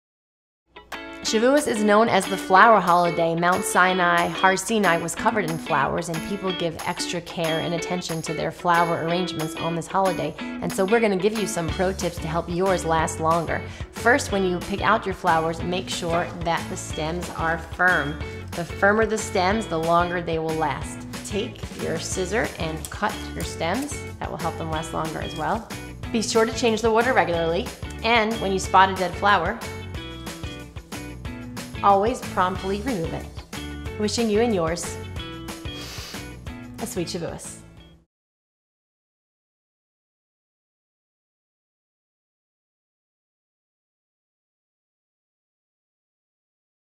Video Hack: Shavuos Fresh Flower Protips
Want to know the secret to having fabulous flowers for Shavuos or anytime? Watch and learn! A special thank you to Chanie Waxler of Buds Designs

Seth Feldman – producer
Danny Moallem – production assistant